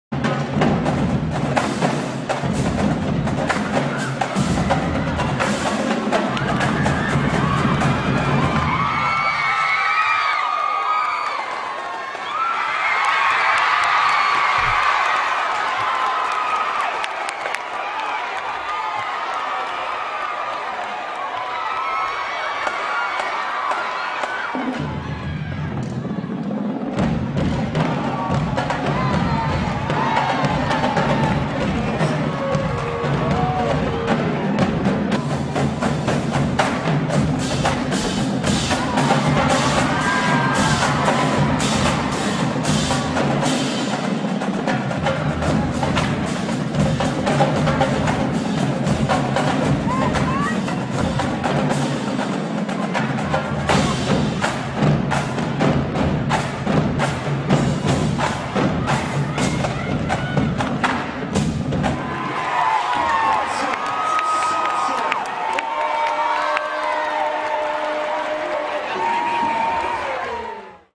Bricyn performing with VSU
Bricyn playing with VSU Nov 2019 (5 years old)